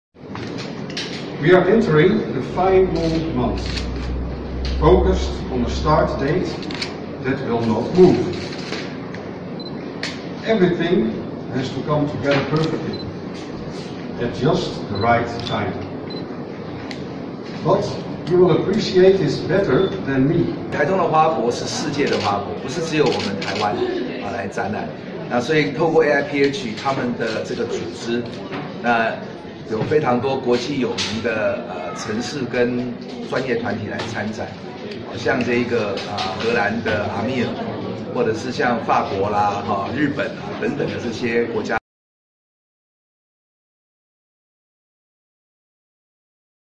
AIPH視察台中花博籌備進度 會長：已聽見花開的聲音
2018台中世界花卉博覽會將於今年11月3日登場，各項工作緊鑼密鼓推展中，國際園藝生產者協會（AIPH）會長Bernard Oosterom、秘書長Tim Briercliffe等人專程來台視察籌備進度，為讓國內外人民了解花博國際競賽項目及精彩看點，花博辦公室26日於東勢客家文化園區召開參展進度說明會，盼透過花博盛會提升台灣園藝產業的國際能見度。